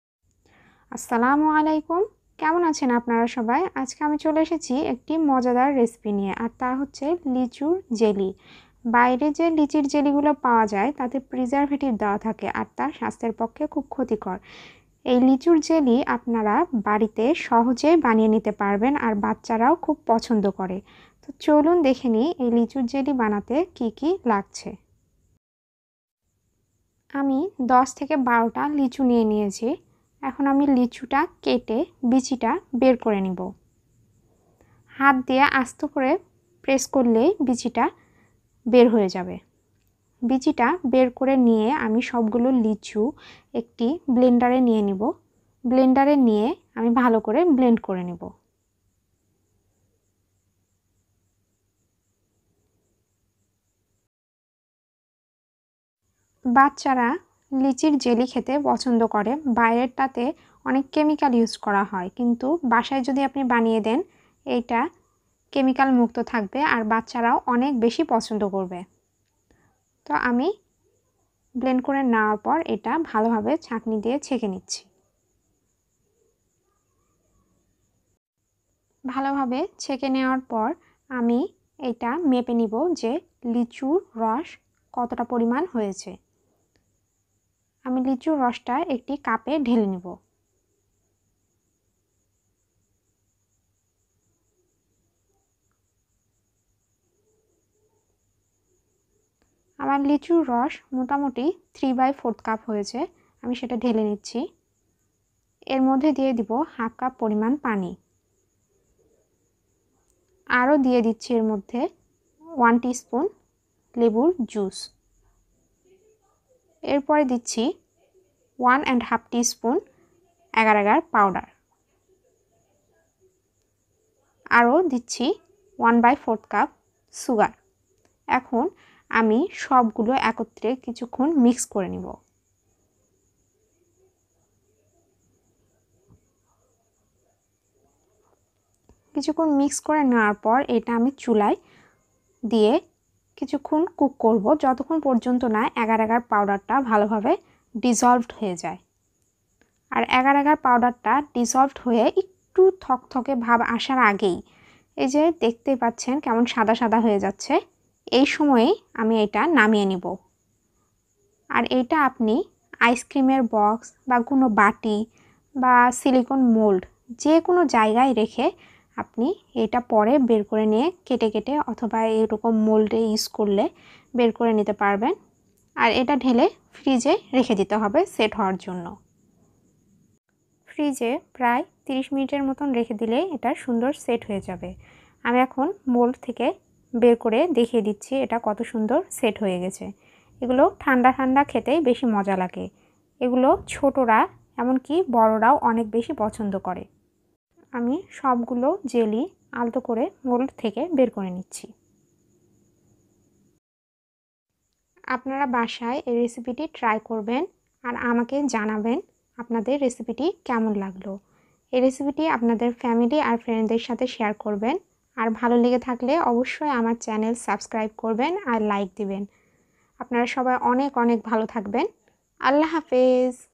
বাসায় সহজেই তৈরি লিচুর জেলি রেসিপি||Homemade litchi jelly recipe
Ingredients
10-12 litchis
1/2 cup water
1 & 1/2 tsp agar agar powder
1/4th cup sugar
1 tsp lemon juice